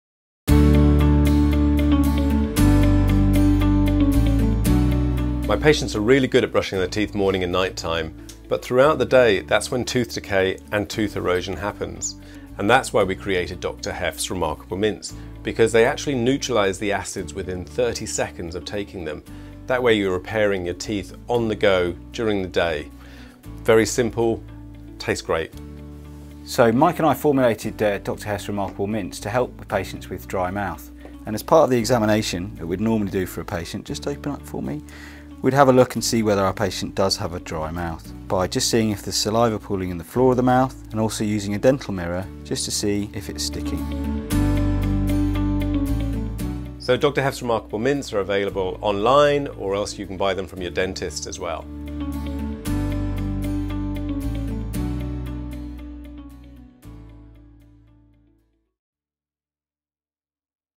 Dr Heff's Remarkable Mints- what are they and how do they work
When ever we go to trade events people always ask what makes them "Remarkable"?
Here Dr Mike Heffernan and Dr. Toby Edwards-Lunn explain why...